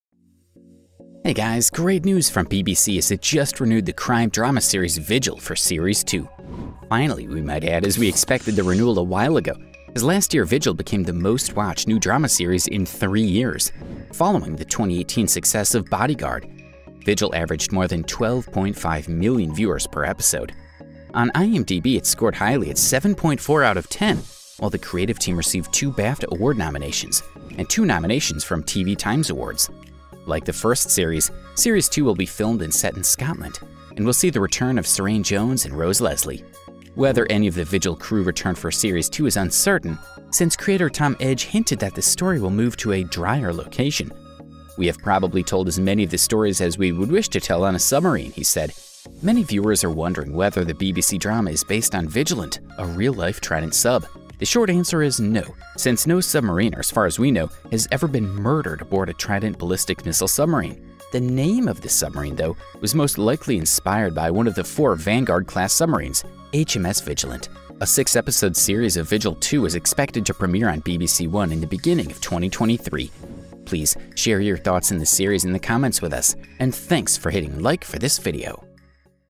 VIGIL Series 2 Confirmed for 2023: Story Moves from the Submarine to the Land, Suranne Jones Reveals
Hey, guys! Great news from BBC as it just renewed the crime drama series Vigil for Series 2. Finally, we might add, as we expected the renewal a while ago, as last year Vigil became the most-watched new drama series in three years (following the 2018 success of Bodyguard). Vigil averaged more than 12.5 million viewers per episode. On IMDb, it’s scored highly at 7.4 out of 10, while the creative team received two BAFTA Award nominations, and two nominations from TV Times Awards. Like the first series, Series 2 will be filmed and set in Scotland, and will see the return of Suranne Jones and Rose Leslie. Whether any of the Vigil crew return for Series 2 is uncertain, since creator Tom Edge hinted that the story will move to a drier location. "We have probably told as many of the stories as we would wish to tell on a submarine," he said. Many viewers are wondering whether the BBC drama is based on Vigilant, a real-life Trident sub. The short answer is ‘no’, since no submariner, as far as we know, has ever been murdered aboard a Trident ballistic missile submarine. The NAME of the submarine, though, was most likely inspired by one of the four Vanguard-class submarines, HMS Vigilant. A 6-episode Series 2 of Vigil is expected to premiere on BBC One in the beginning of 2023. Please, share your thoughts on the series in the comments with us.